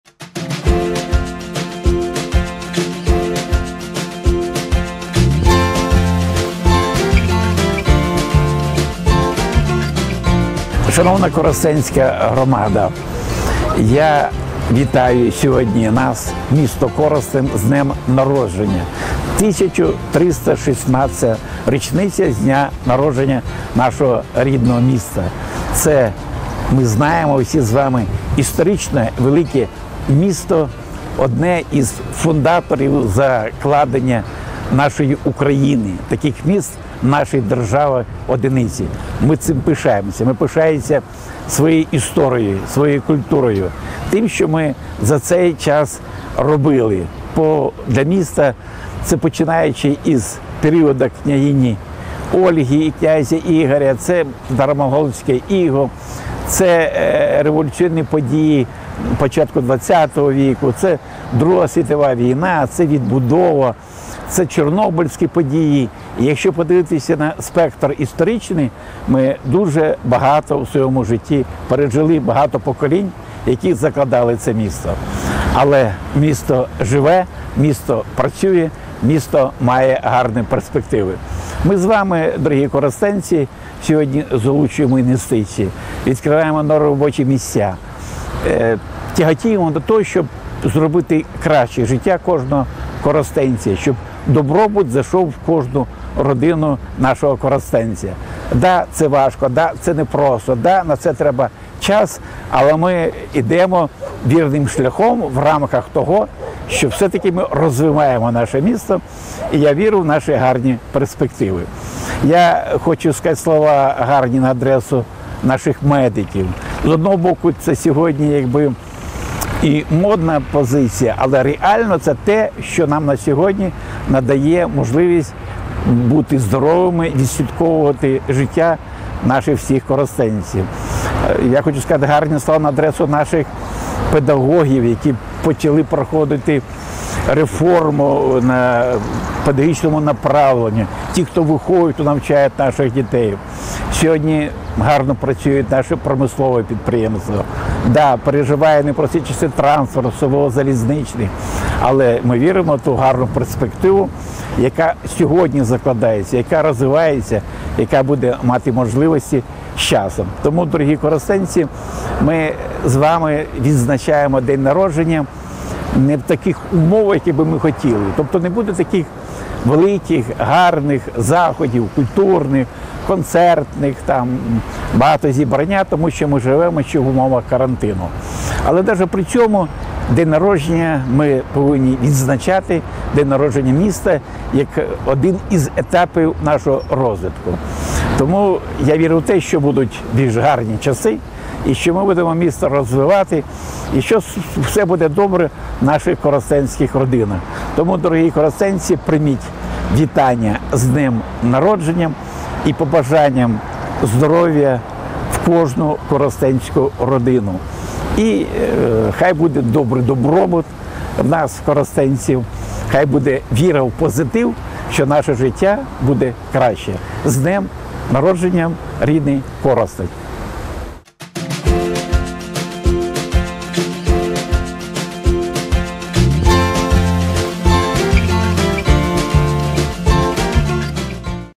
KorostenTV_29-05-21_З Днем міста від Володимира Москаленка
Коростенську громаду з Днем міста вітає міський голова Коростеня Володимир Москаленко.